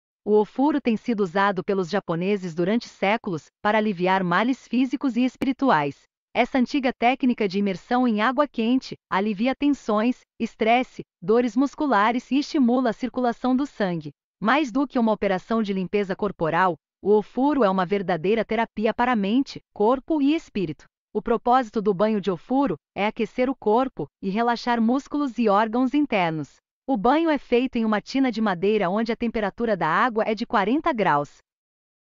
Banho de Ofuro
Conheça os benefícios do banho de ofuro!
Alívio de dores musculares,  melhoria na qualidade do sono, uma verdadeira terapia para alma e para o corpo
Ligue Hidroviana Bali